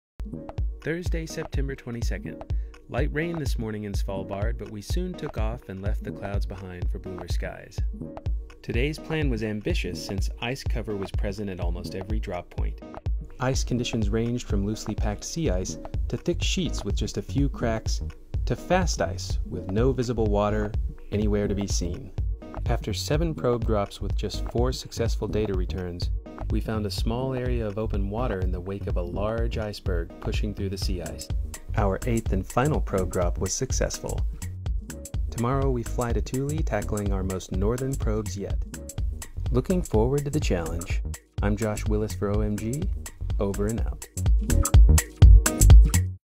Oceans Melting Greenland (OMG) Field Campaign: Vlog #1
On September 22, 2016, OMG Principal Investigator Josh Willis of NASA’s Jet Propulsion Laboratory reported from the field in Greenland.